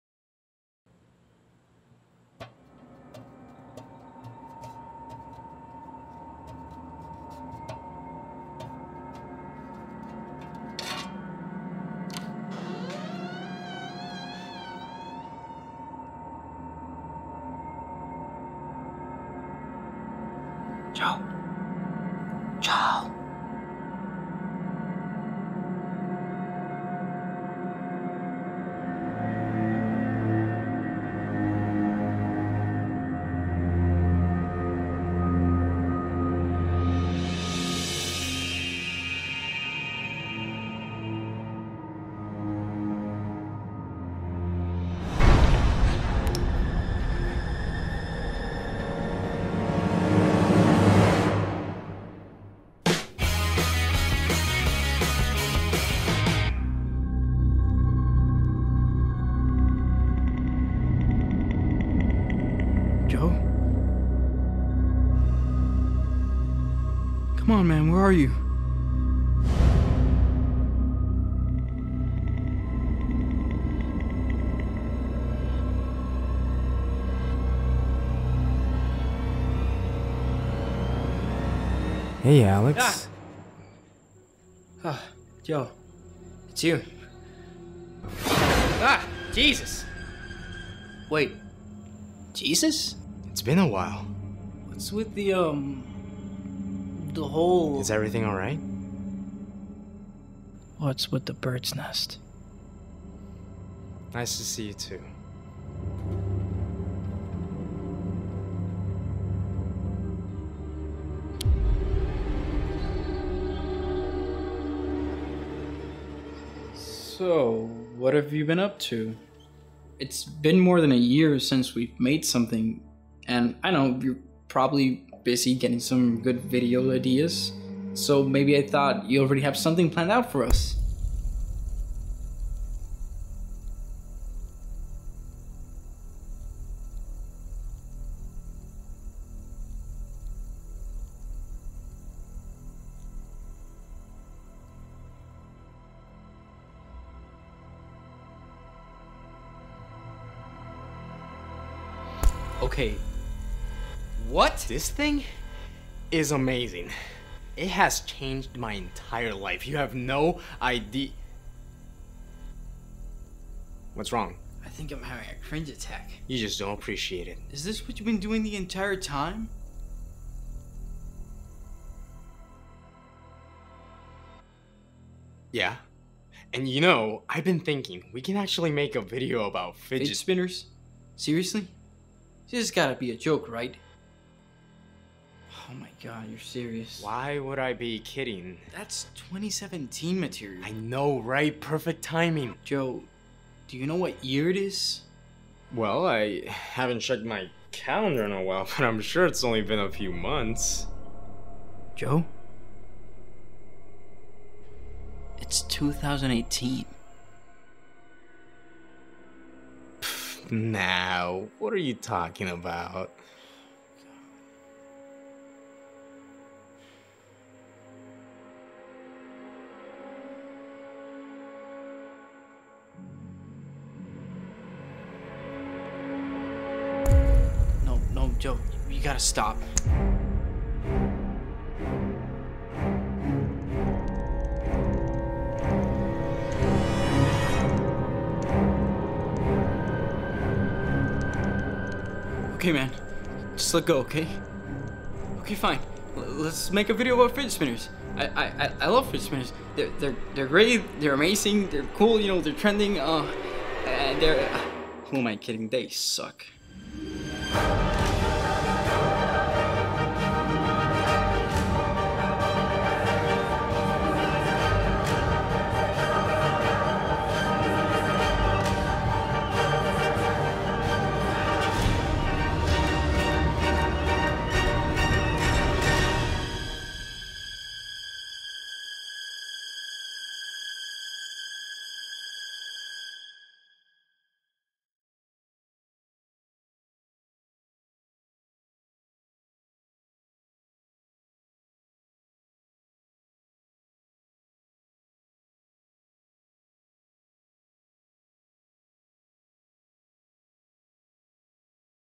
The Cursed Trend
It seems Joe has been possessed by an old cursed trend this whole time...

Starring:
•Alex Diaz (@aj15mp)
•Joe Rodriguez (@joewgram)

Sound:
•Andres Rodriguez (@dj_yofro1)